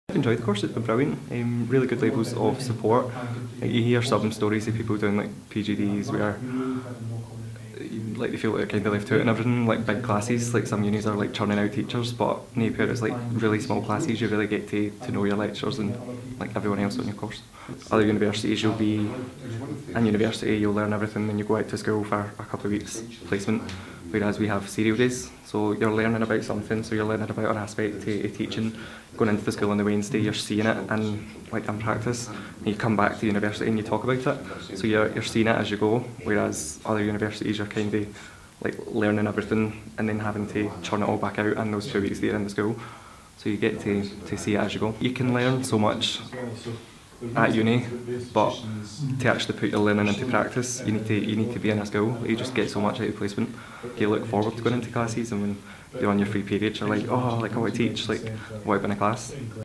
Edinburgh Napier University | SAS | Bradley; Life as a biology teacher on placement
We talk to Bradley Mackie, a PGDE Biology student at Edinburgh Napier University. He explains to us what it's like to be on placement teaching in a school and how it compares to other universities.

Edinburgh Napier University
School of Applied Sciences (SAS)
Professional Graduate Diploma in Education Biology

This video was uploaded by John 'Jay' Carrington, Brand & Reputation Content Intern